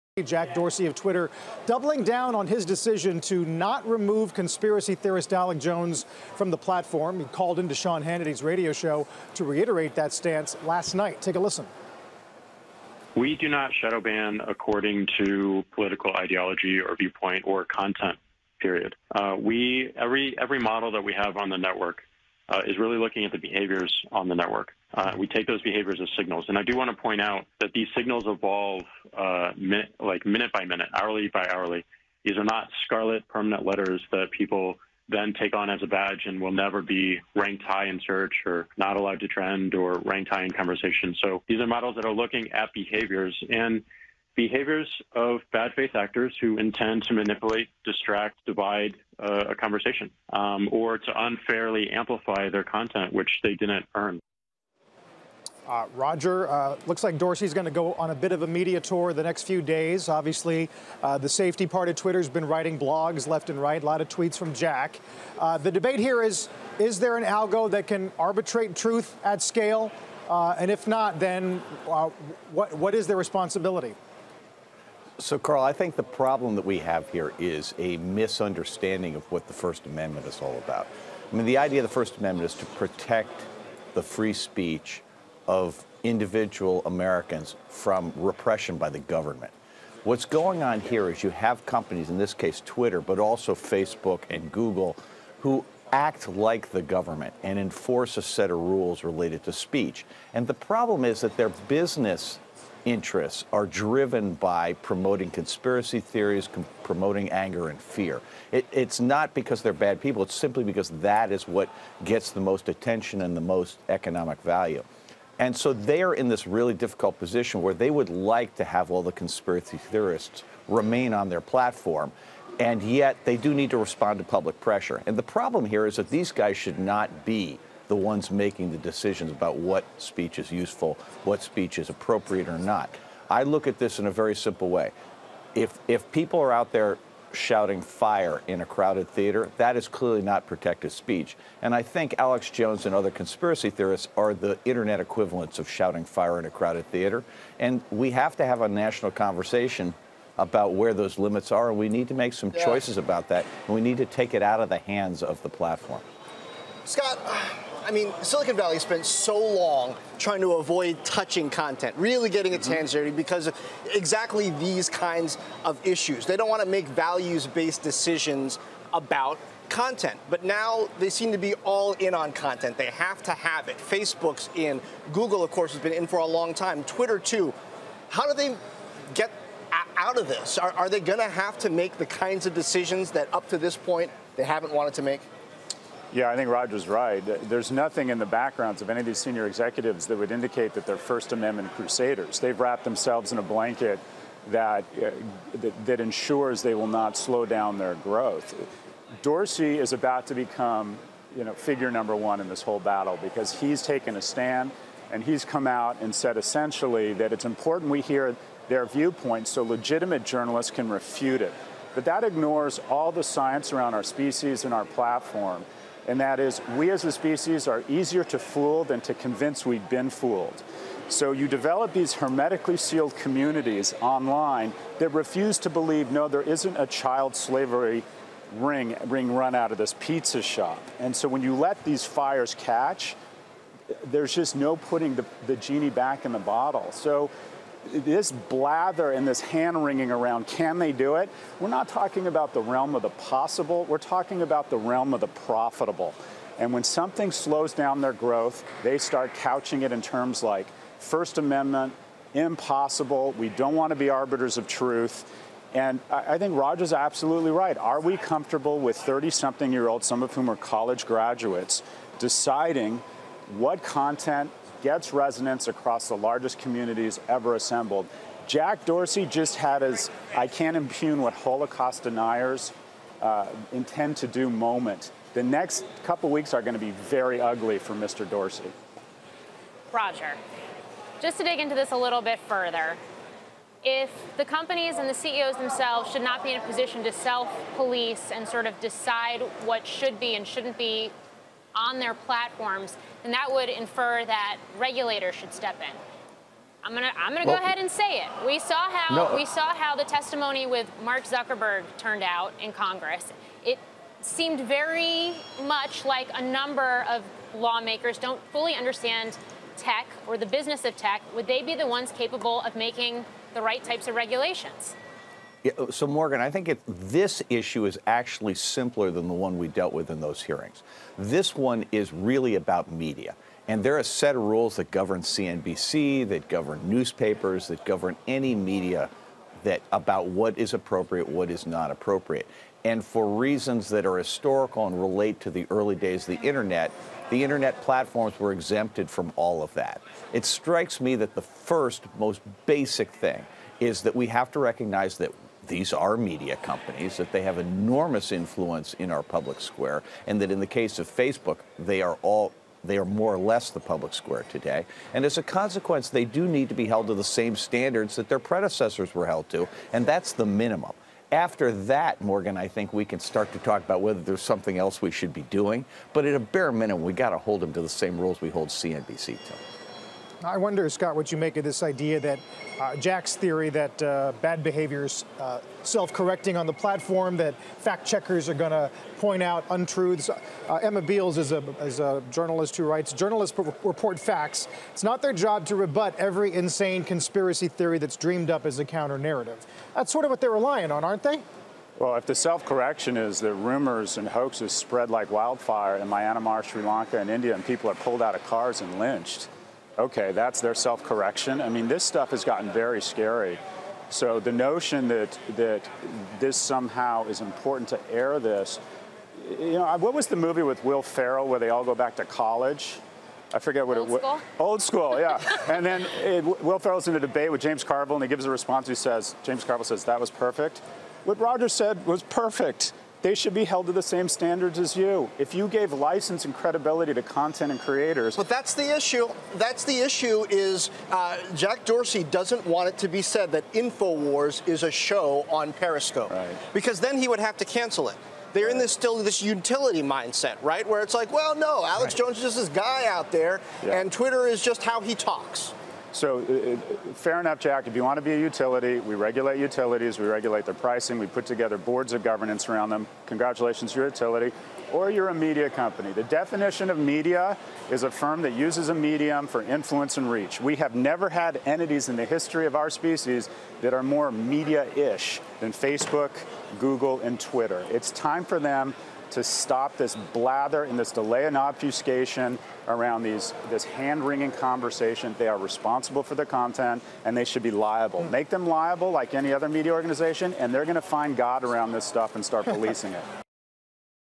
Should social platforms be regulated?
Experts discuss why they think social platforms should be regulated like media companies